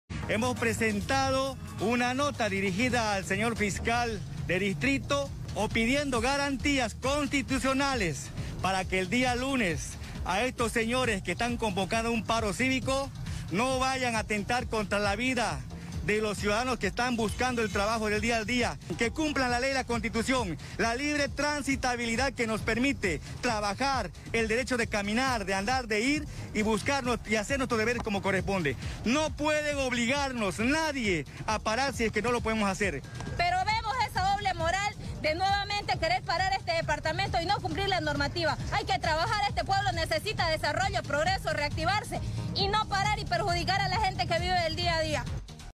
𝗠𝗮𝘀𝗶𝘀𝘁𝗮𝘀 𝗽𝗶𝗱𝗲𝗻 𝗹𝗮𝘀 𝗴𝗮𝗿𝗮𝗻𝘁𝗶́𝗮𝘀 𝗽𝗮𝗿𝗮 𝗰𝗶𝗿𝗰𝘂𝗹𝗮𝗿 𝗲𝗻 𝗽𝗮𝗿𝗼 𝗾𝘂𝗲 𝗲𝗹𝗹𝗼𝘀 𝗻𝗼 𝗱𝗮𝗻 𝗰𝘂𝗮𝗻𝗱𝗼 𝗯𝗹𝗼𝗾𝘂𝗲𝗮𝗻 𝗦𝗮𝗻𝘁𝗮 𝗖𝗿𝘂𝘇.
𝗠𝗮𝘀𝗶𝘀𝘁𝗮𝘀 𝗽𝗶𝗱𝗲𝗻 𝗹𝗮𝘀 𝗴𝗮𝗿𝗮𝗻𝘁𝗶́𝗮𝘀 𝗽𝗮𝗿𝗮 𝗰𝗶𝗿𝗰𝘂𝗹𝗮𝗿 𝗲𝗻 𝗽𝗮𝗿𝗼 𝗾𝘂𝗲 𝗲𝗹𝗹𝗼𝘀 𝗻𝗼 𝗱𝗮𝗻 𝗰𝘂𝗮𝗻𝗱𝗼 𝗯𝗹𝗼𝗾𝘂𝗲𝗮𝗻 𝘆 𝗰𝗲𝗿𝗰𝗮𝗻 𝗦𝗮𝗻𝘁𝗮 𝗖𝗿𝘂𝘇.

Tras la ratificación del paro Cívico en Santa Cruz por vía del Comité Interinstitucional por el Censo, con una actitud en extremo cínica e hipócrita, asambleístas y concejales del Movimiento al Socialismo (MAS-IPSP), recurren al Ministerio Público para pedir al Fiscal departamental, les brinde garantías para poder circular el día del paro departamental del lunes 25 de julio, algo que ellos jamás dan cuando deciden cercar y bloquear Santa Cruz a su antojo de forma sistemática, por turnos de organizaciones sociales masistas y por razones absurdas o antojadizas, sin importarles el daño económico que le producen al país y está olvidando la misma reactivación económica.

Los masistas, recién ahora enuncian el derecho constitucional al libre tránsito que ellos no observan ni respetan en lo más mínimo cuándo bloquean cualquier parte o todo el departamento Cruceño; también, defendiendo el derecho al trabajo de los ciudadanos que a ellos no les importa para nada cuando ejecutan acciones de bloqueos agresivos contra Santa Cruz, cómo ha sucedido en estos últimos meses, con fines absolutamente políticos y de presunto odio racial extremo contra el camba cruceño, olvidando que todos y muy especialmente los que vienen del interior del país a vivir en Santa Cruz, se benefician con mejor acceso a la salud, a la educación y de muchas otras formas, gracias a los resultados del censo, algo qué ahora el régimen masista a la cabeza del dictador Luis Arce Catacora, violando la Constitución Política el Estado (CPE), qué prevé que el censo se tiene que hacer cada 10 años, pretende postergar hasta el 2024.